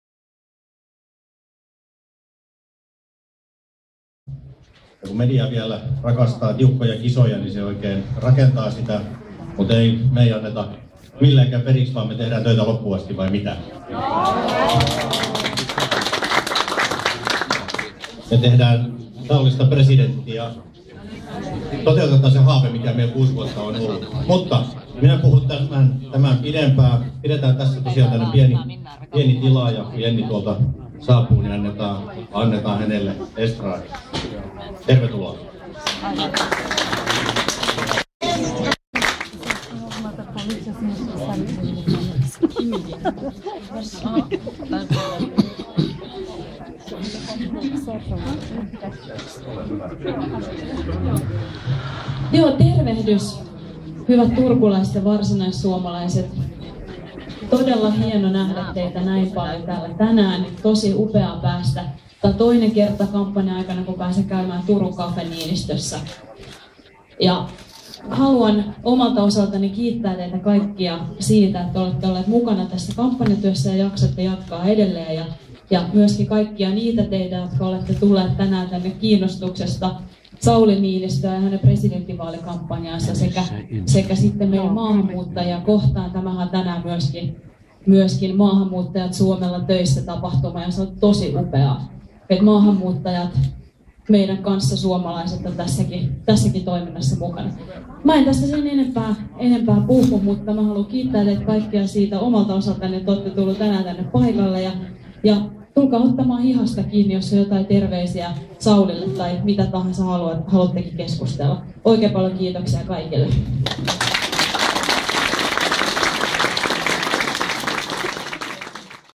First Lady of The Republic of Finland - Jenni Haukio / Nokia N8 HD videotest 27.01.2012
First Lady of The Republic of Finland - Jenni Haukio at Cafe Niinistö Turku Finland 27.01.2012 / Nokia N8 HD videotest